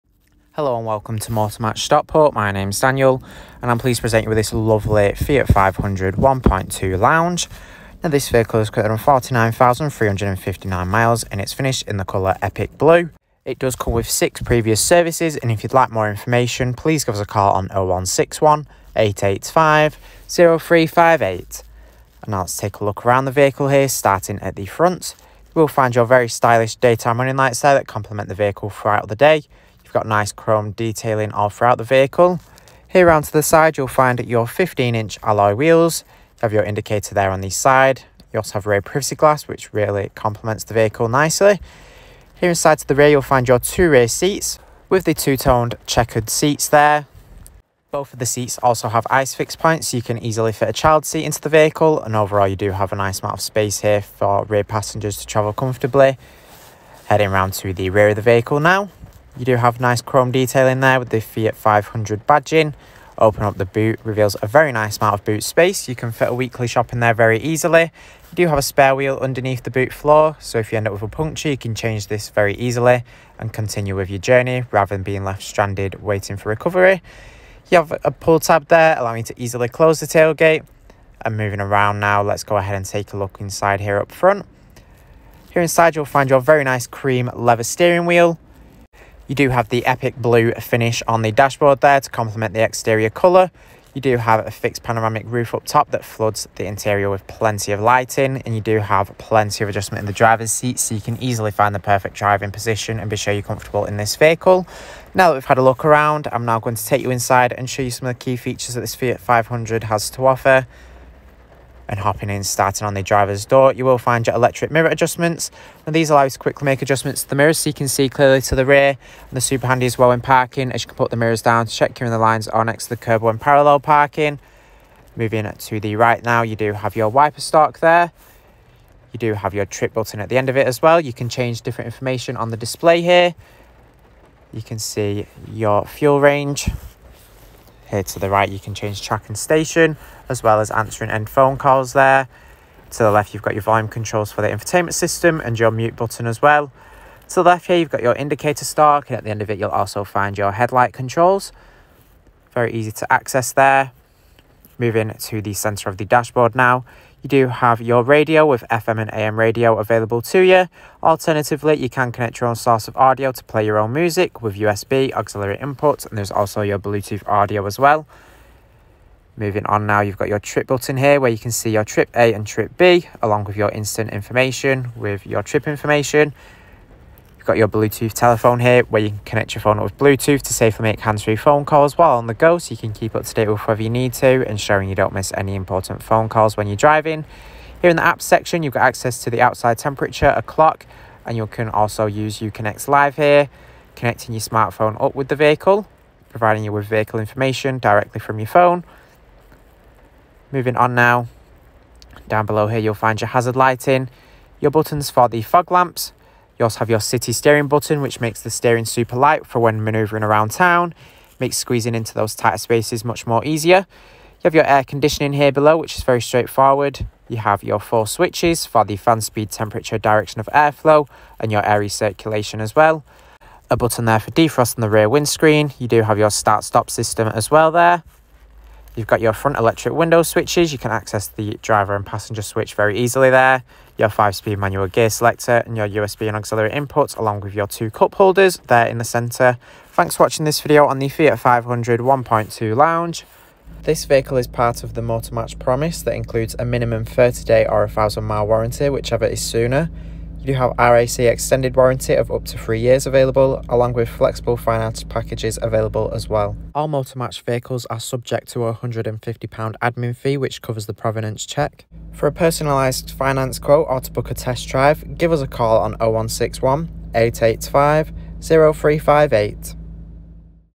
Approved Used Fiat 500 1.2 Lounge | Motor Match Stockport
Just arrived in stock this Fiat 500 1.2 Lounge is now available for sale from Motor Match Stockport.

Reg - NU65 ZFO
Mileage - 49,359
Transmission - Manual
Fuel - Petrol